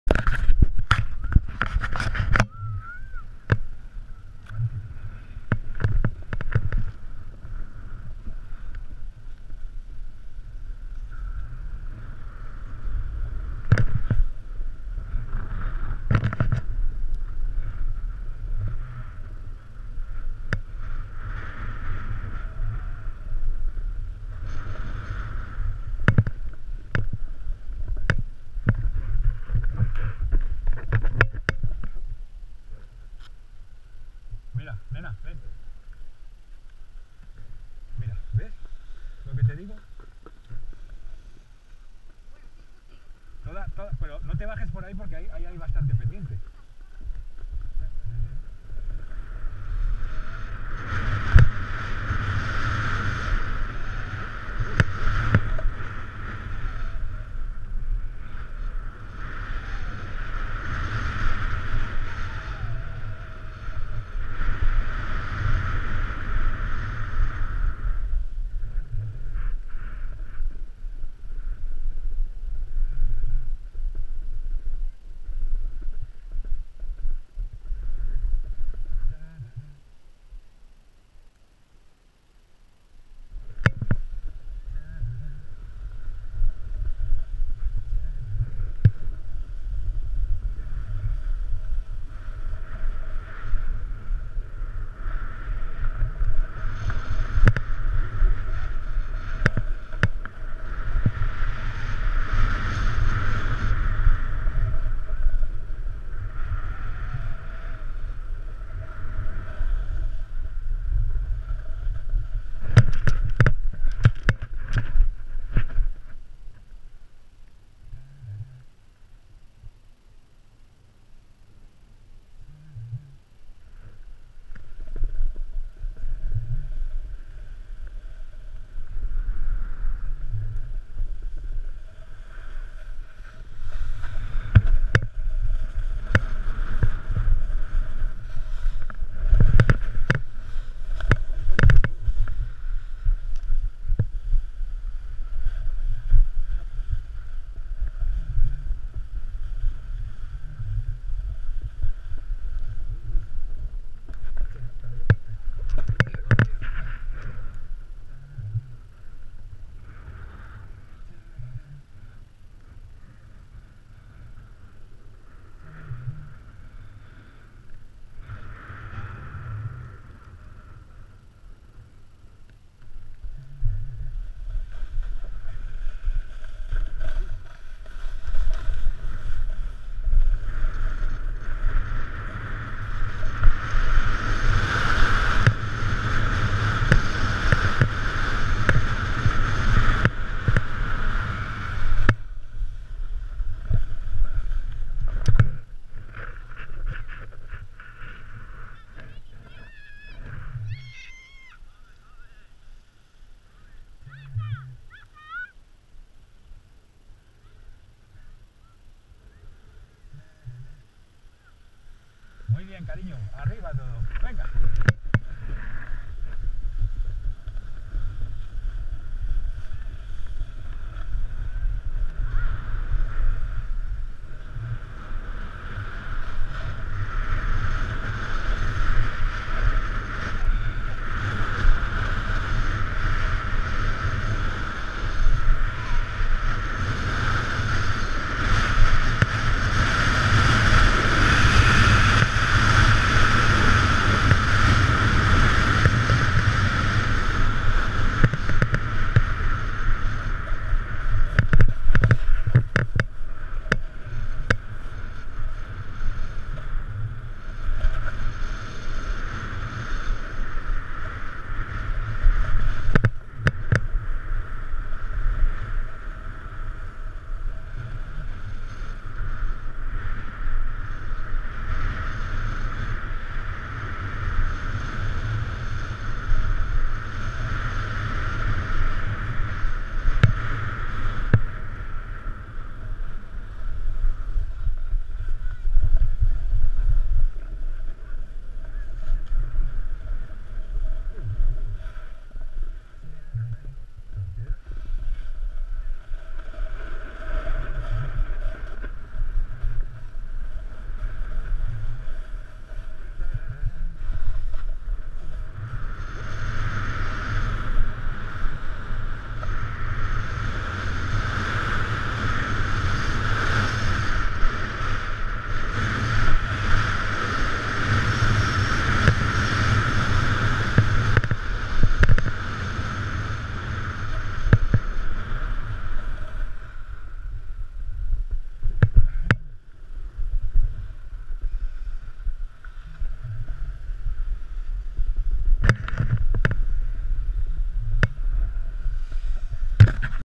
Bajada por Canal Roya a enlazar con Rinconada, Formigal
Comenzamos esta bajada un poco antes de lo que sería el punto exacto desde el que comienza la pista Canal Roya, por aquello de abreviar el pateo que hay que hacer, enlazando con la pista Rinconada.